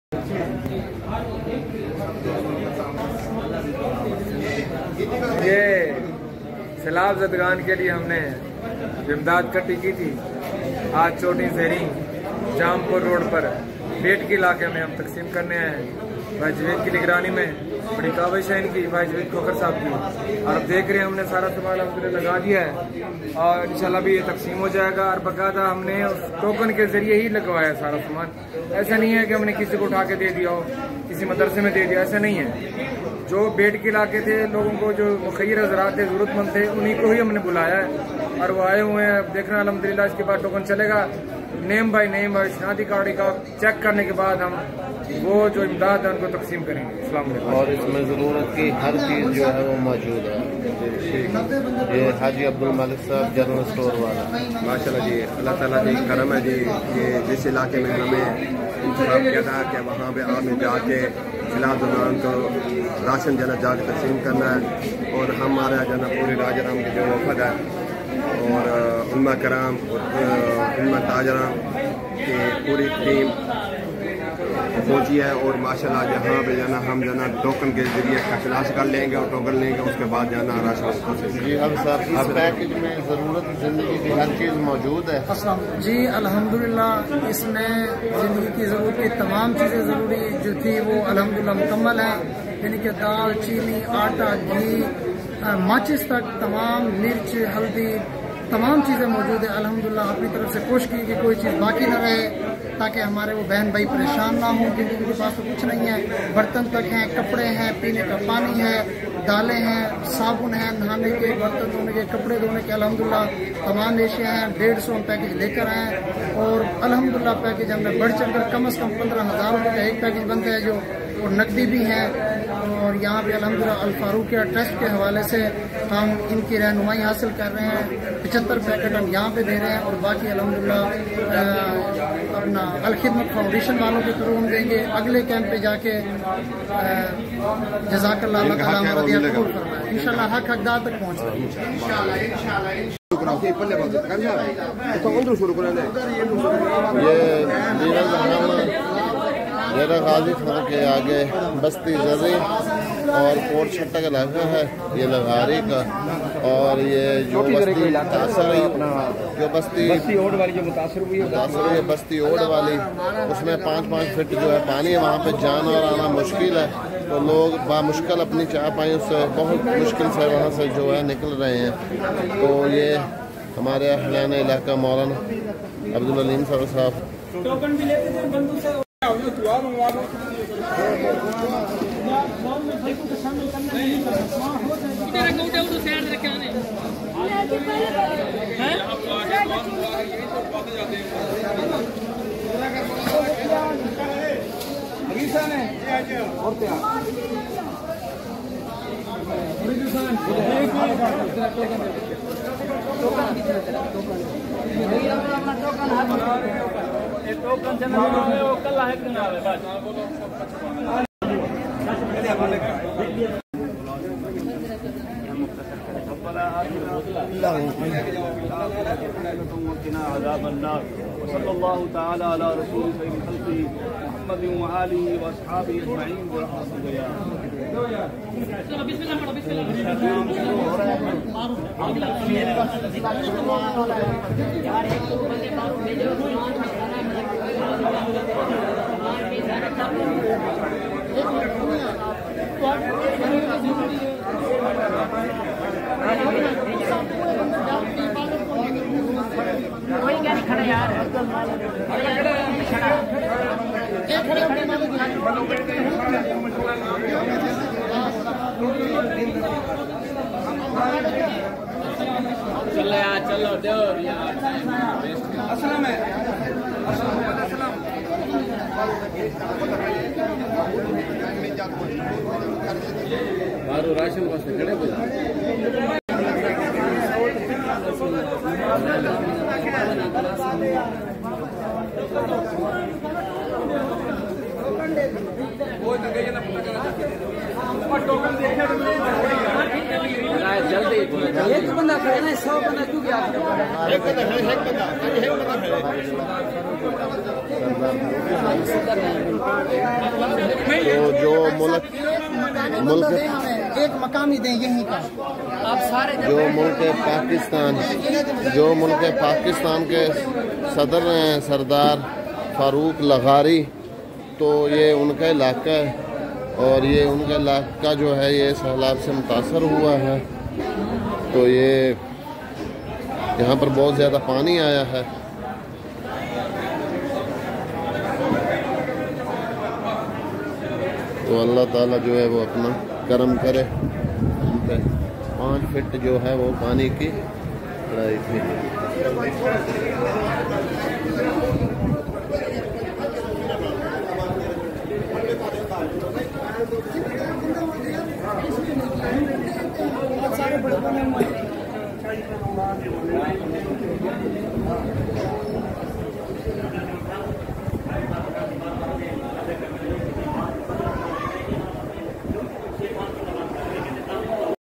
Flood Relief funds distributed in DG Khan areas | Anjuman Tajran Zareef Shaheed
flood relief camp 🏕 in DGK